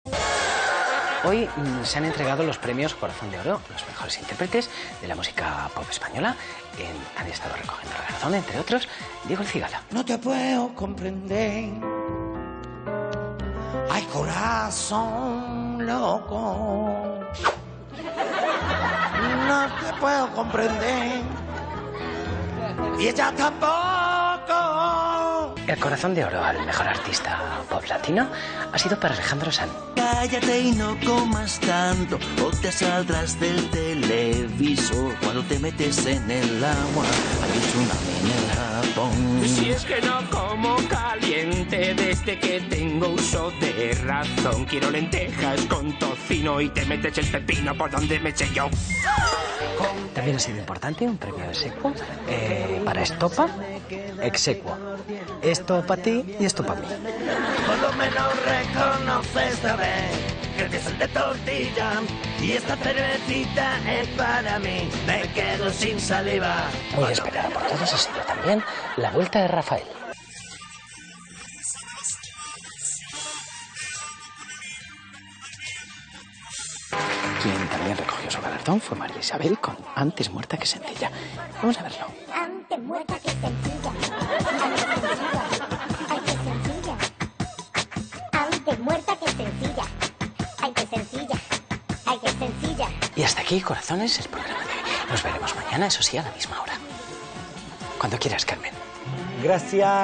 Anne Igartiburu da paso a los ganadores del 'Corazón de oro'
Parodía del 'Corazón de Otoño' donde Anne Igartiburu da a conocer a los artistas ganadores del 'Corazón de Oro'